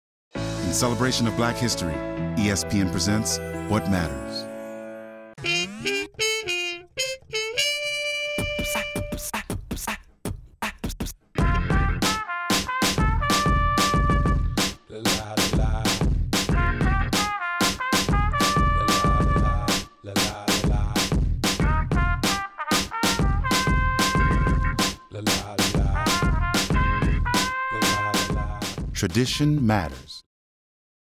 ESPN "What Matters?" Basketball spot
Directed by Alastair Christopher. Part of ESPN "What Matters?" campaign created by Amani Martin